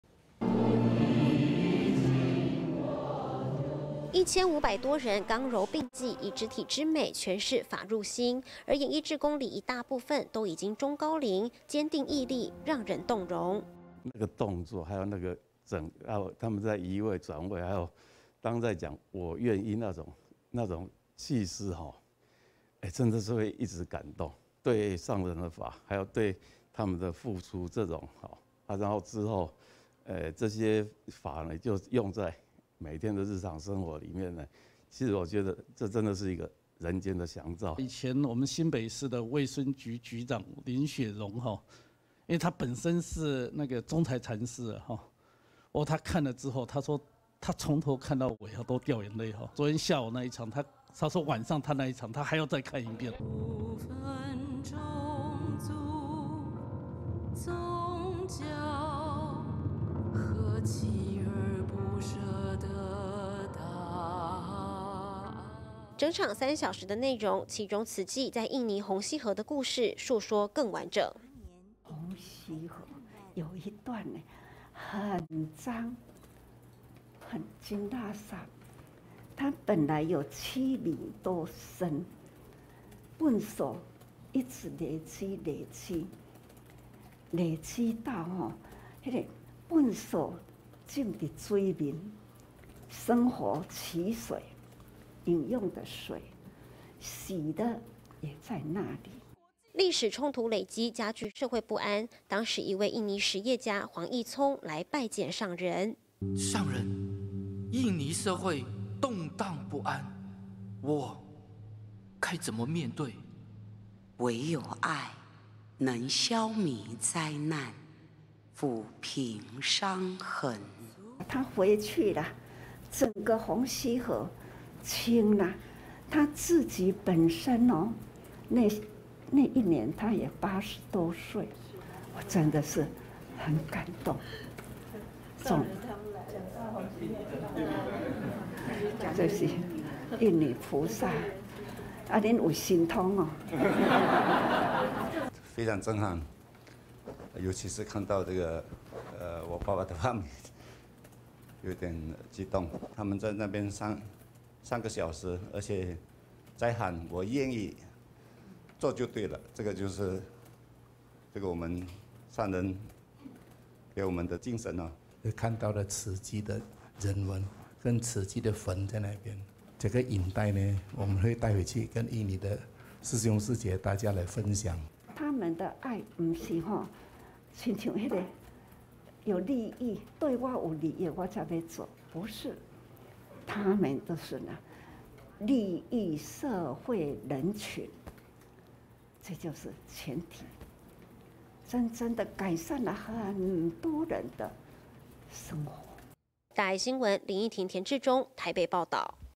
演繹重現篳路藍縷 菩薩足跡步生蓮
北區經藏演繹，昨天第七場，上人親自到場觀賞，今天早上，行腳行程，上人開示看完演繹的感動，現場也來了北慈的醫師，以及慈濟印尼分會兩位副執行長。看到演繹將歷史重現，也表示那分震憾和慈濟精神，銘刻在心。

1500多人剛柔並濟，以肢體之美詮釋法入心，而演繹志工裡一大部分，都已經中高齡，堅定毅力讓人動容。

台北慈濟醫院副院長 張恒嘉：「那個動作還有那個移位轉位，還有他們在講 我願意那種氣勢，真的是會一直感動，對 上人的法 還有對他們的付出，然後之後 些法又用在，每天的日常生活裡面，其實我覺得這真的是一個，人間的祥兆。」

台北慈濟醫院副院長 徐榮源：「以前我們新北市的，衛生局局長林雪蓉，因為她本身是中台禪寺的，不過她看了之後她說，她從頭看到尾她都掉眼淚，昨天下午那一場(看完)，她說晚上那一場她還要再看一遍。」

整場三小時的內容，其中慈濟在印尼、紅溪河的故事述說更完整。

證嚴上人開示：「紅溪河有一段很髒，它本來有七米多深，垃圾一直累積累積，累積到那個垃圾浸在水面，生活取水飲用的水洗的也在那裡。」

歷史衝突累積，加劇社會不安，當時一位印尼企業家黃奕聰來拜見 上人。

「 上人印尼社會動盪不安，我該怎麼面對，唯有愛能消弭災難撫平傷痕。」

證嚴上人開示：「他回去了 整個紅溪河 清啊，他自己本身 那一年，他也80多歲 我真的是很感動，(上人他們來了)，就是印尼菩薩 你們有神通喔。」

慈濟印尼分會副執行長 黃榮年：「非常震撼尤其是看到這個，我爸爸 有點激動，他們(演繹志工)在那邊三個小時，而且在喊我願意做就對了，這個就是我們上人給我們的精神。」

慈濟印尼分會副執行長 郭再源：「看到了慈濟的人文，跟慈濟的魂在那邊，這個影帶 我們會帶回去，跟印尼的師兄師姊大家來分享。」

證嚴上人開示：「他們的愛不是像有利益，對我有利益我才要做不是，他們都是利益社會人群，這就是前提，真正地改善了很多人的生活。」

台北報導：林亦庭 田智中